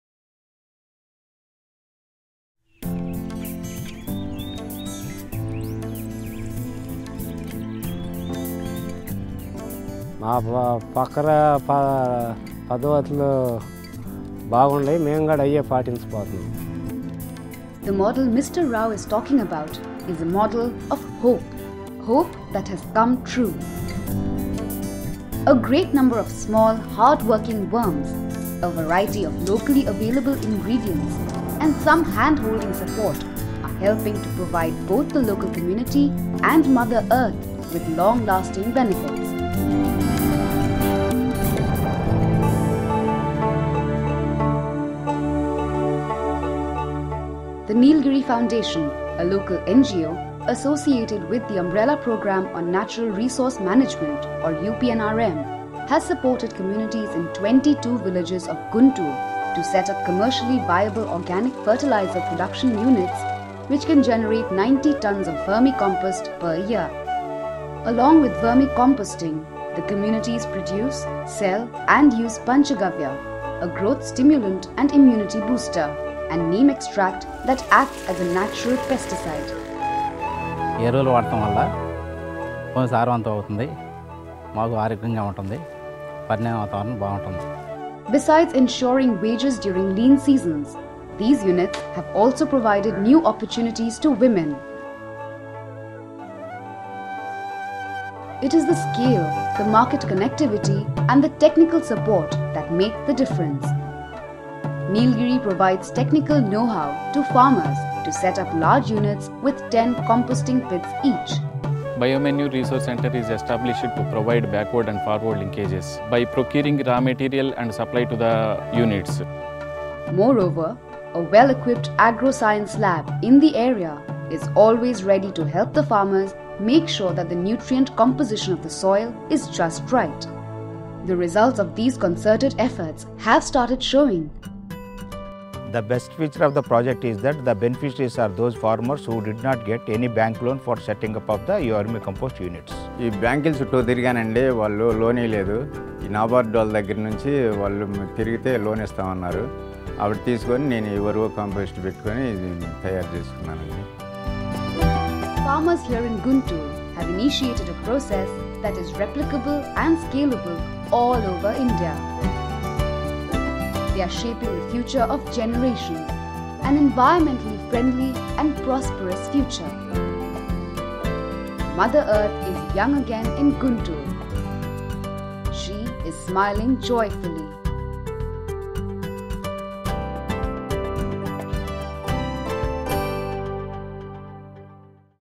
Ground Truths. Nilagiri ( Vermicomposting )
The financial and environmental cost of using chemical pesticides is high. The project introduces 50 poor families to a commercially viable alternative - residue free, organic agriculture. Substituting chemical pesticides with organic manure makes agri-business more profitable while reducing environmental and health risks for surrounding community.